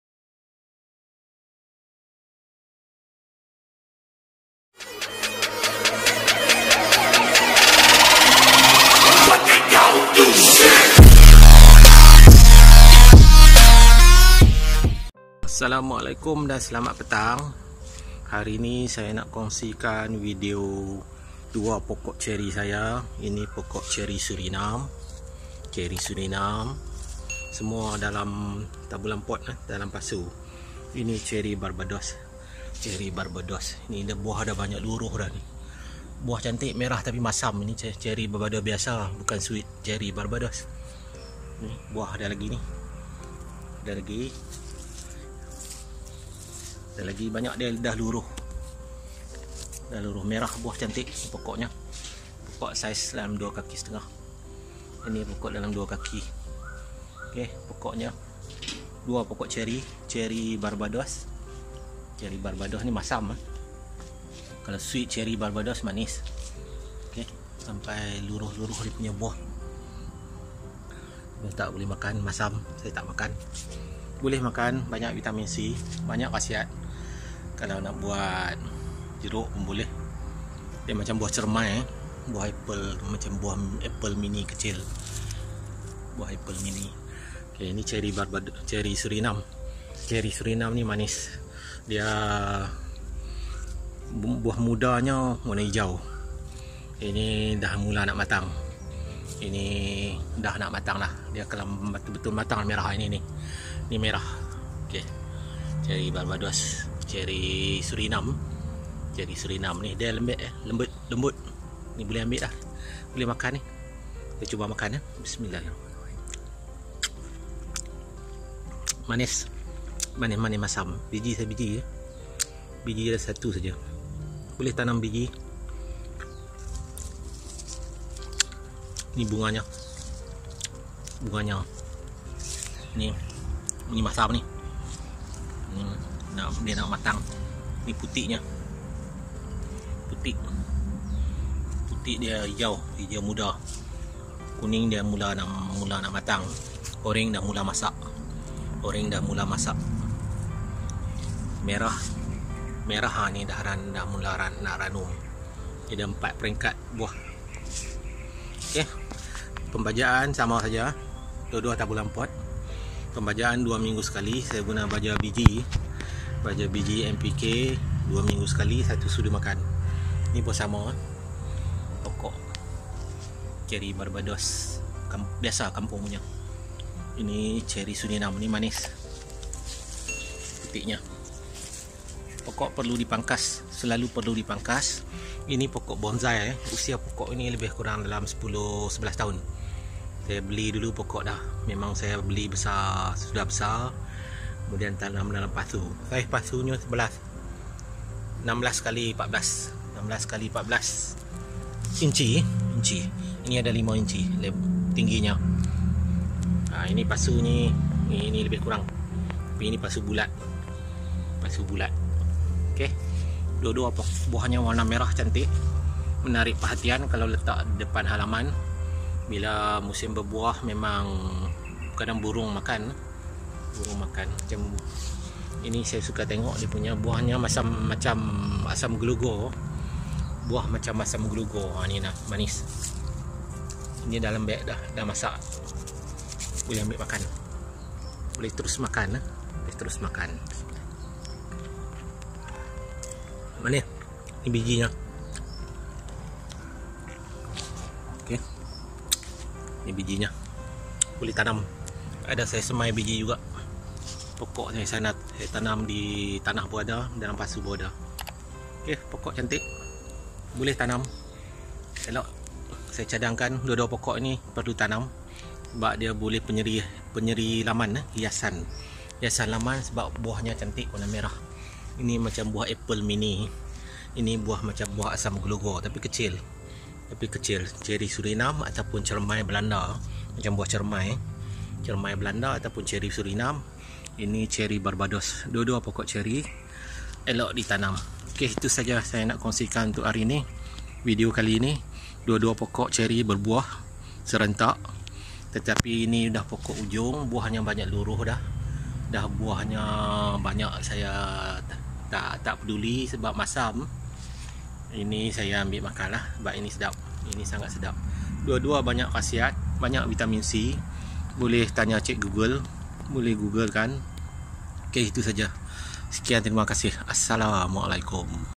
CERI BARBADOS VS CERI SURINAM@CERMAI BELANDA @ DEWANDARU | POKOK CHERRY
Pokok ceri barbados dan ceri surinam TABULAMPOT (TAnam BUah daLAM POT) sedang berbuah lebat. Kedua-dua buahnya berwarna merah menyala. Sesuai sebagai pokok hiasan. Sangat sesuai ditanam di dalam pasu dan dijadikan pokok bonsai.